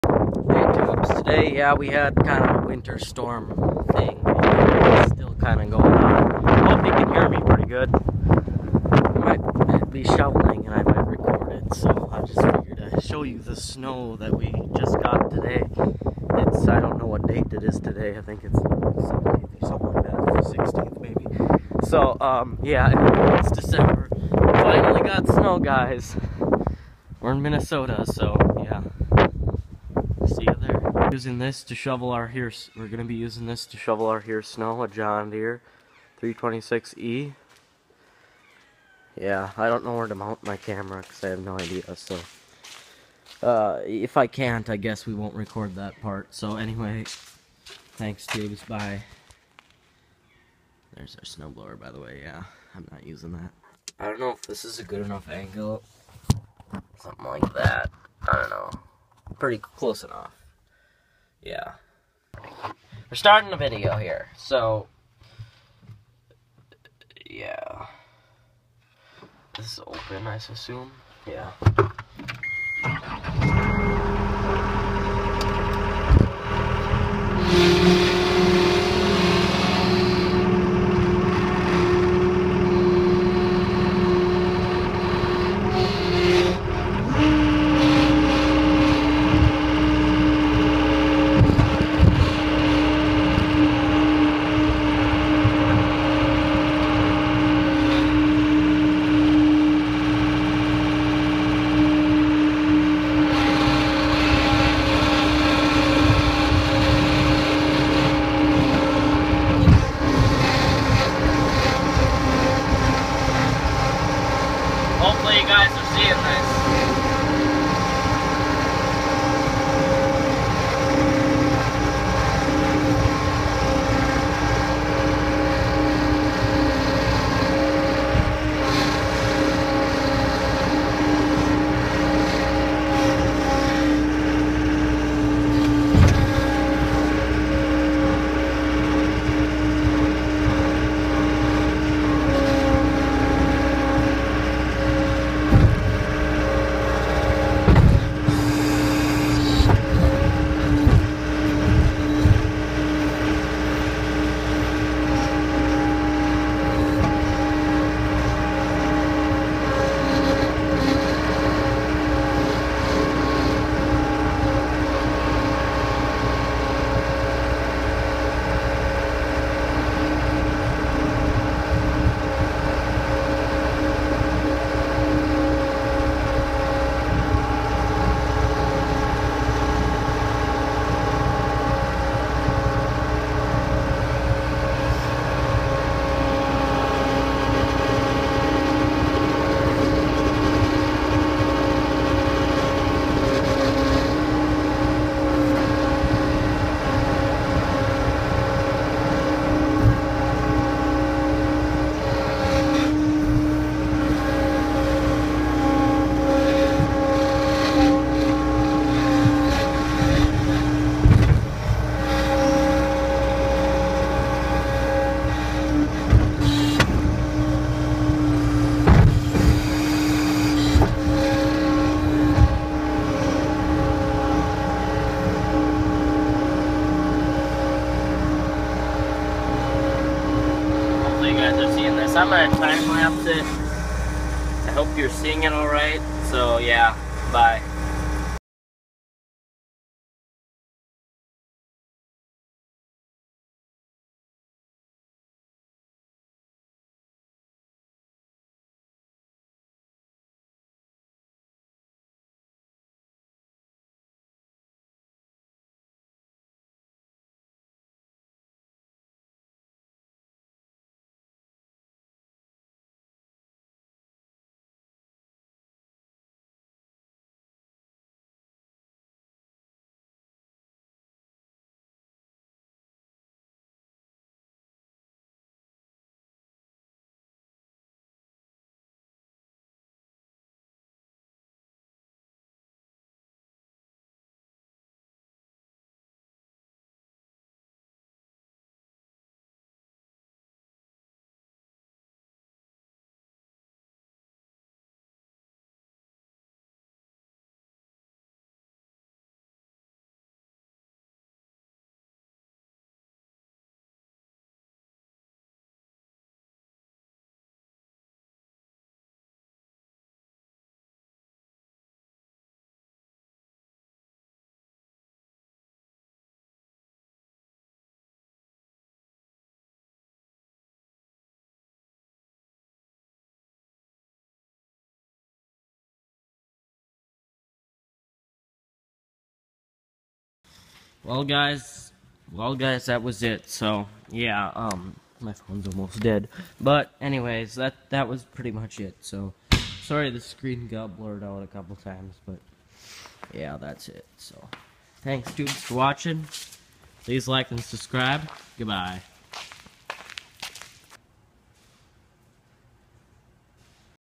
Shoveling Snow!!! 2015 John Deere
Sorry if the cammera keeps tipping back
And sorry about the snow that blocked the view in some parts well anyway tubes dont forget to
BILLSTMAXX2!